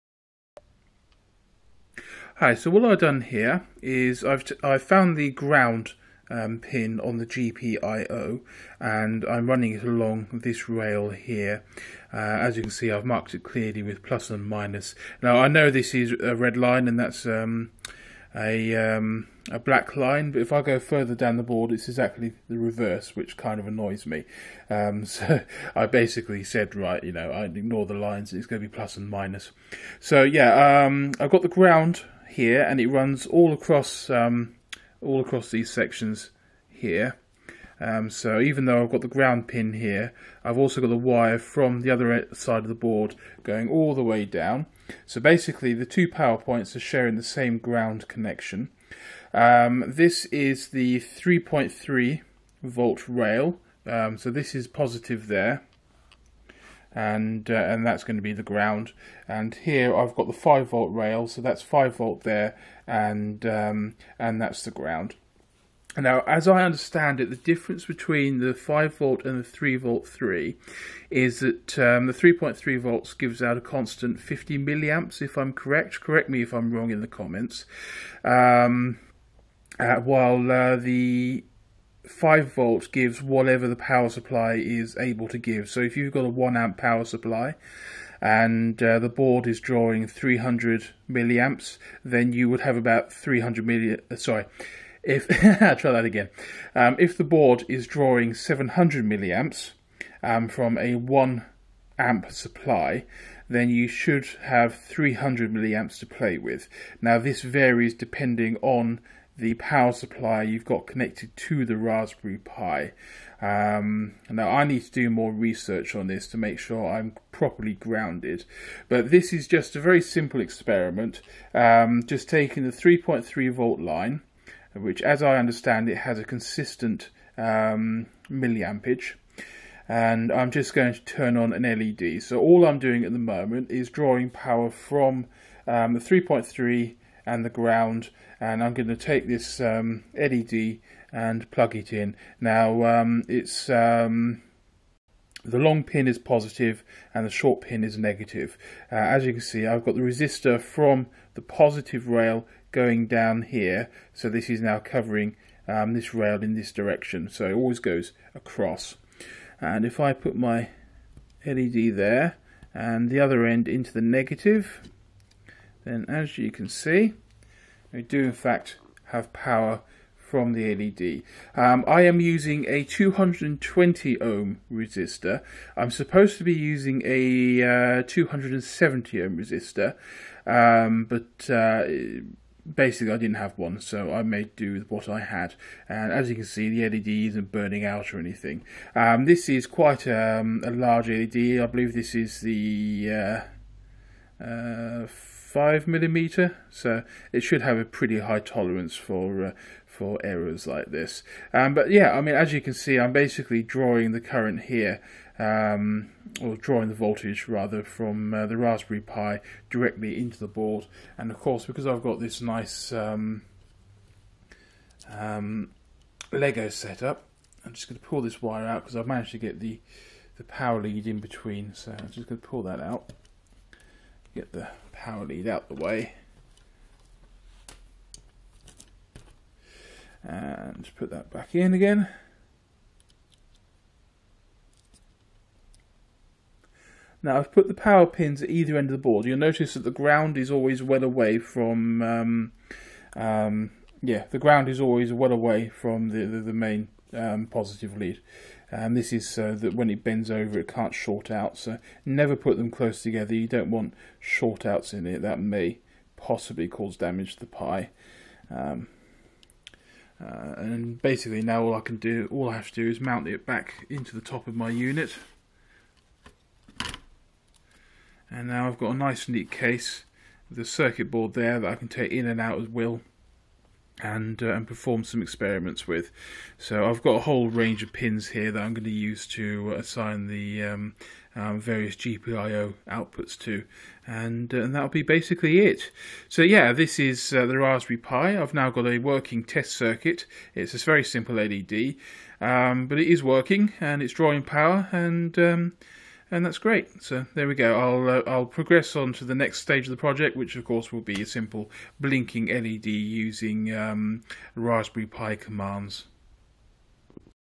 Raspberry Pi Diaries #3 - simple LED circuit via GPIO output
In this video I demonstrate a working LED output from the Raspberry Pi's 3.3 volt output and a breadboard using the new lego based case. I also discuss the 5V power output.